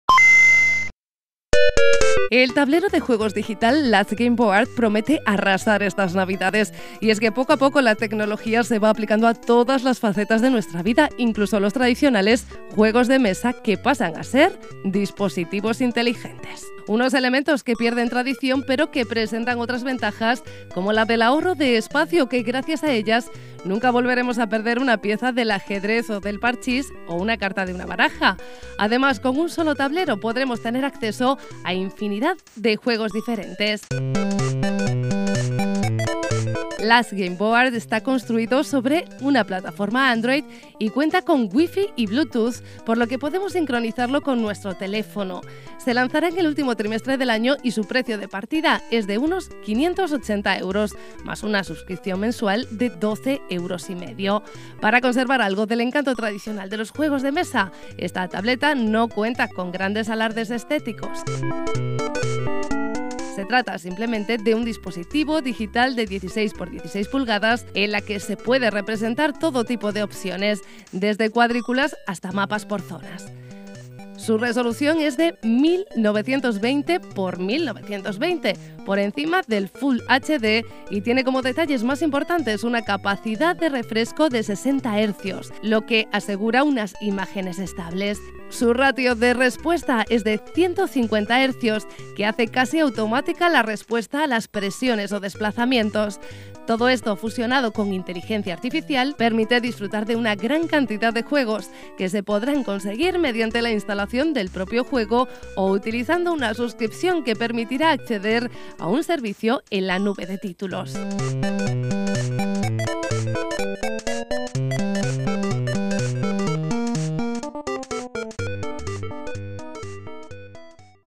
LUJO Y TECNOLOGÍA-MESA DE JUEGOS DIGITAL | By CdRas
El tablero de juegos digital Last Gameboard promete arrasar estas navidades. Y es que poco a poco la tecnología se va aplicando a todas las facetas de nuestra vida, incluso a los tradicionales juegos de mesa que pasan a ser dispositivos inteligentes.

Unos elementos que pierden tradición pero que presentan otras ventajas como la del ahorro de espacio o que gracias a ellas nunca volveremos a perder una pieza del ajedrez o del parchís o una carta de una baraja. Además con un solo tablero podremos tener acceso a infinidad de juegos diferentes.

Last Gameboard está construido sobre una plataforma Android y cuenta con Wi-FI y Bluetooth por lo que podemos sincronizarlo con nuestro teléfono. Se lanzará en el último trimestre del año y su precio de partida es de unos 580 euros, más una suscripción mensual de 12,5 euros. Para conservar algo del encanto tradicional de los juegos de mesa,  esta tableta no cuenta con grandes alardes estéticos.

Se trata simplemente de un dispositivo digital de 16x16 pulgadas en la que se puede representar todo tipo de opciones, desde cuadrículas hasta mapas por zonas. Su resolución es de 1.920 x 1.920, por encima de Full HD, y tiene como detalles más importantes una capacidad de  refresco de 60 Herzios, lo que asegura unas imágenes estables. Su ratio de respuesta es de 150 Herzios, que hace casi automática la respuesta a las presiones o desplazamientos. Todo esto, fusionado con Inteligencia Artificial, permite disfrutar de una gran cantidad de juegos que se podrán conseguir mediante la instalación del propio juego o utilizando una suscripción que permitirá acceder a un servicio en la nube de títulos.